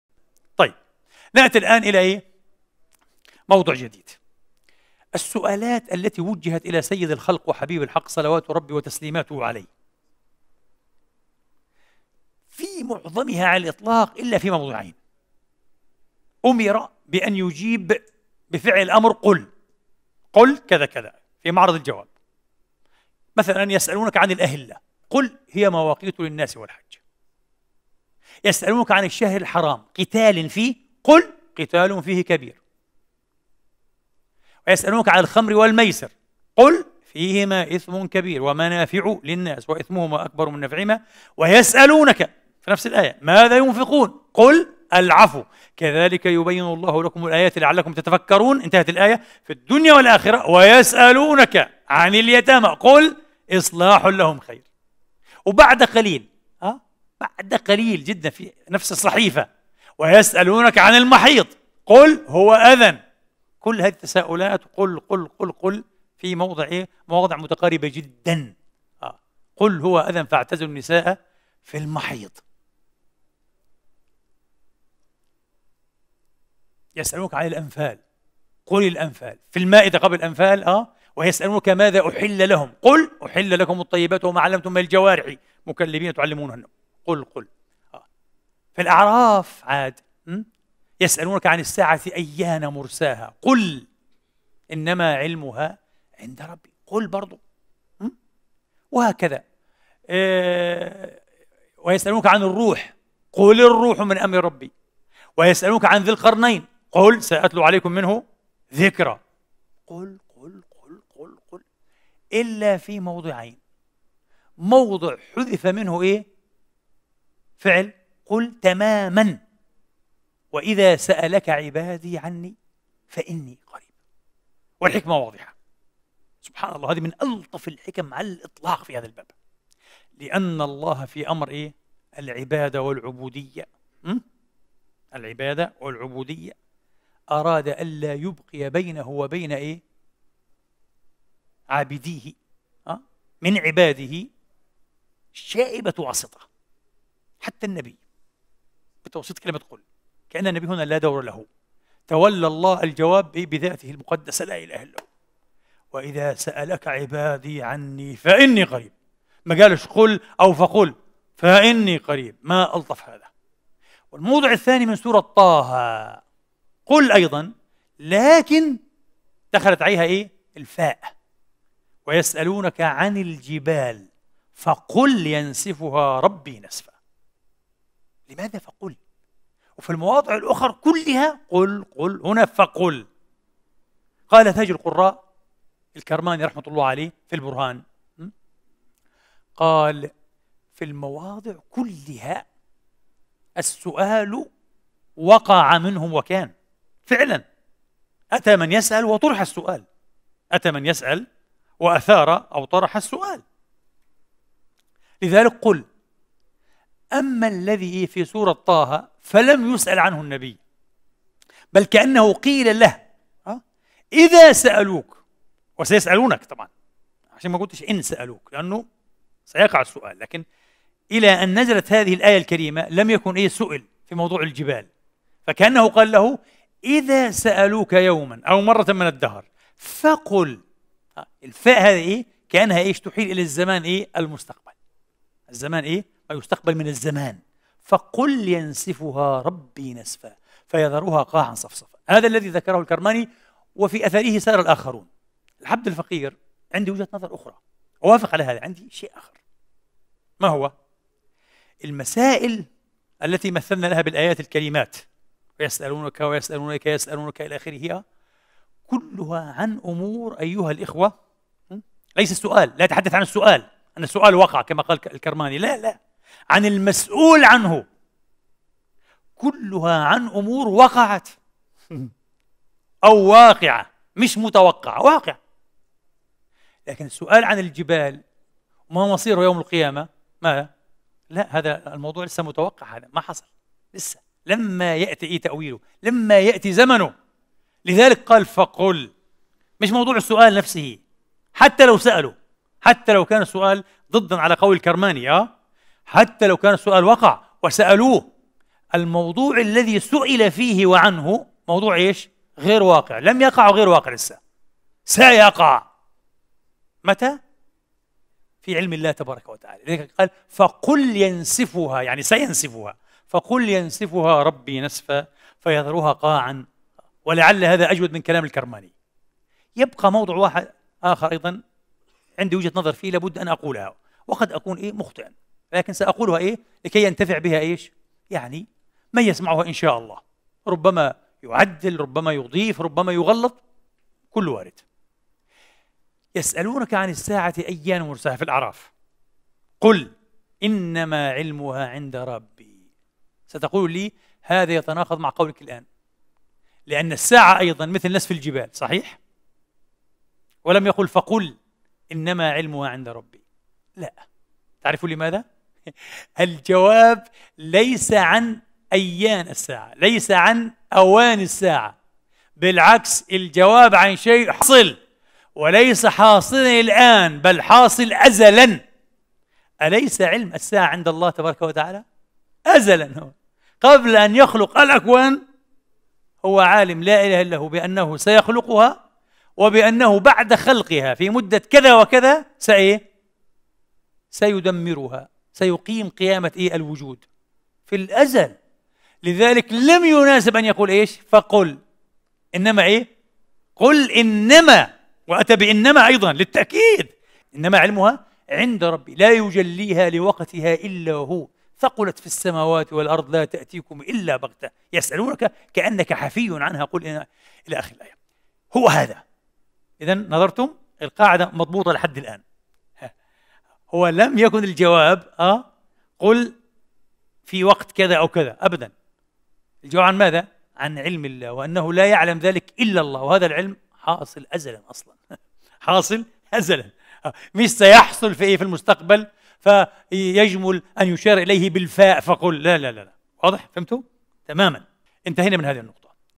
عجيب 🔥 الله قال في كل تساؤلات النبي كلمة قل 🔥 ما عدا موضعين 🔥 مرة لم يقل قل، ومرة قال فقل 🔥 لماذا؟
د.
د.
د.